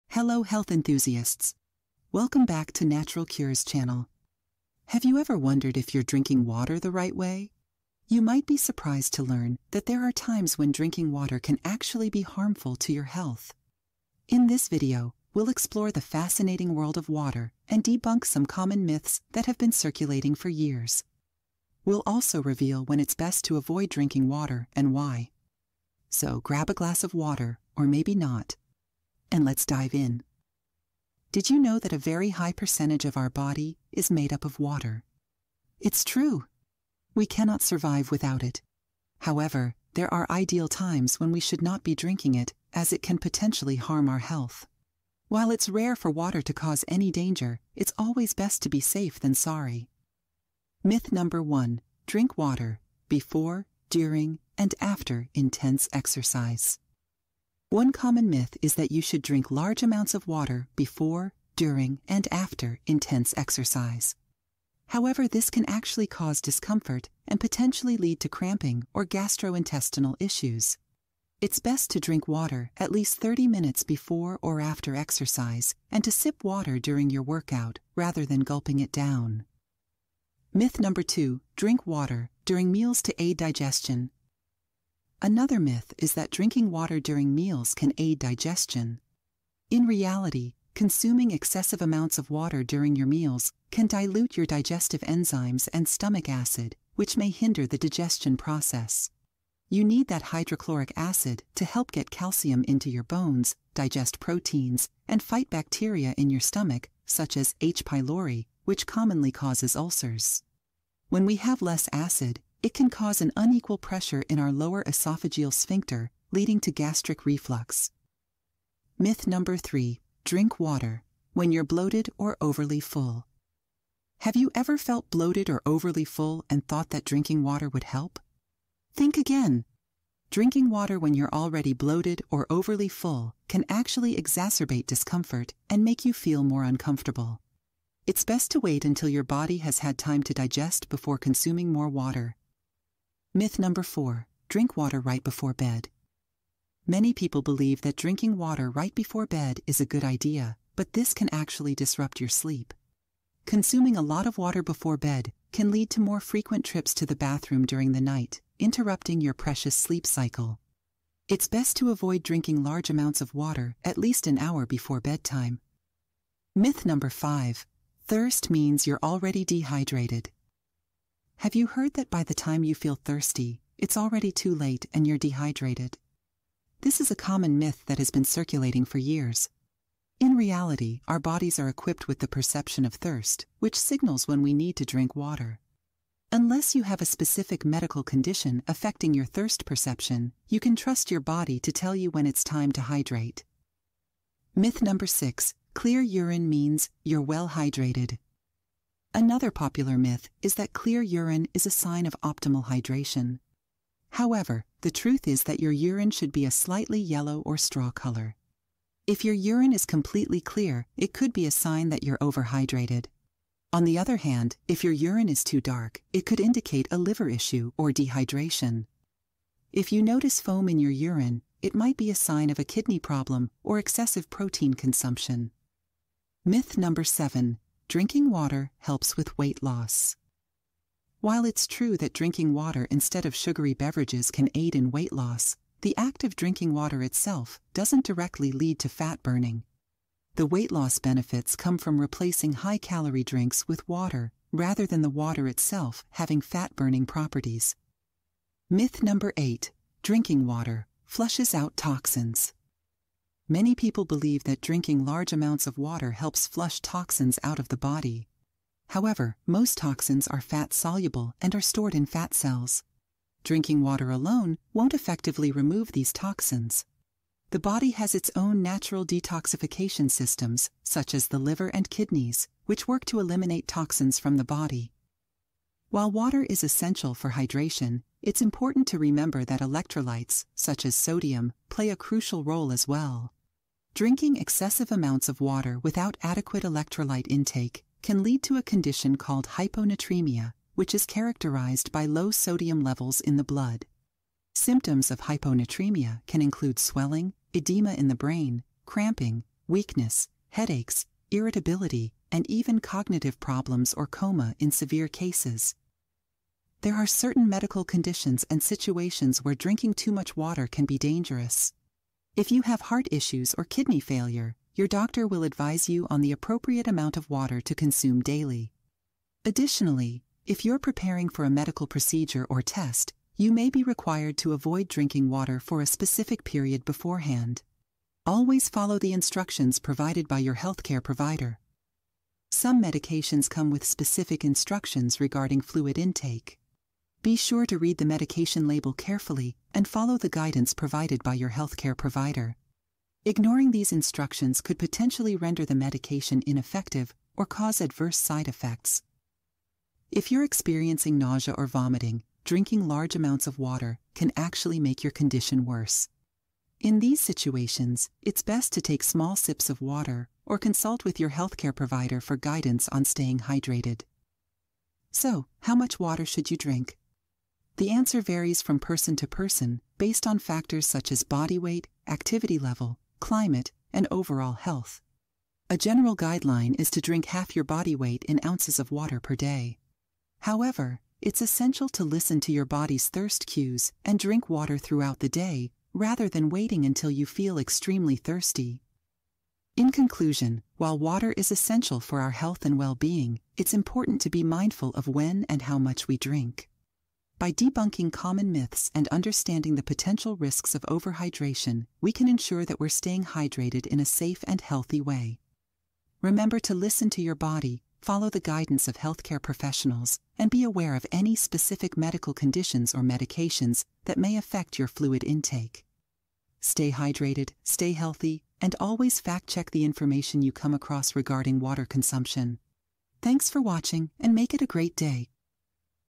8 Mistakes You're Making When Drinking Water
Are you hydrating the right way or falling for common water myths?

Discover the truth in our eye-opening video! We're diving into the dos and don'ts of water consumption, revealing when drinking water might not be the best idea for your health.

From exercise tips to mealtime habits and debunking the thirst myth, we've got the facts that might just change the way you drink.

00:00 Introduction
00:50 Myth #1 - Water and exercise
01:20 Myth #2 - Water during meals
01:58 Myth #3 - Drinking water when bloated
02:23 Myth #4 - Drinking water before bed
02:46 Myth #5 - Thirst and dehydration
03:14 Myth #6 - Urine and hydration
03:46 Myth #7 - Water and weight loss
04:08 Myth #8 - Water flushes out toxins
04:35 Risks of excessive water intake
05:06 Water intake and medical conditions
05:50 Additional tips
06:05 How much water should you drink?
06:29 Conclusion